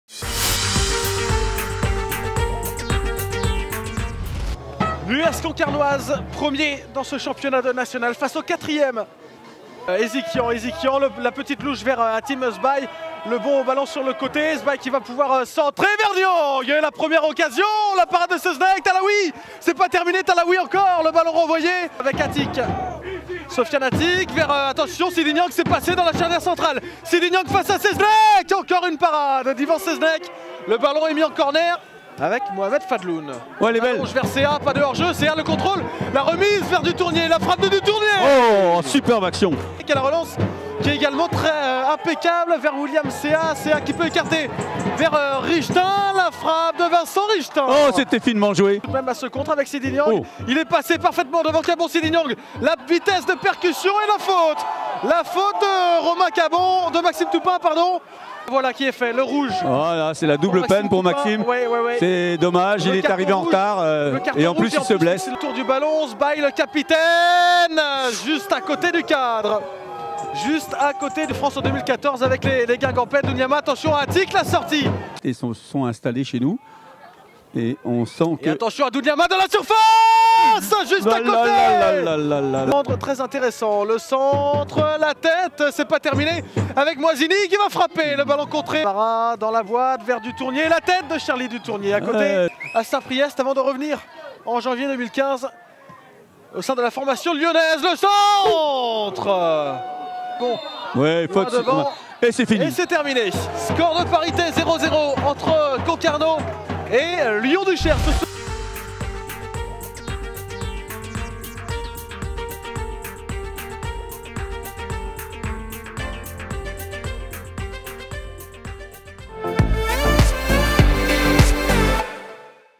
J19 : US Concarneau - Lyon Duchère AS (0-0), le résumé
Réduit à dix après l'exclusion de Mohamed Fadhloun (57e), le leader a réussi à conserver sa cage inviolée malgré plusieurs opportunités lyonnaises. Un match nul qui permet aux Bretons de rester premiers.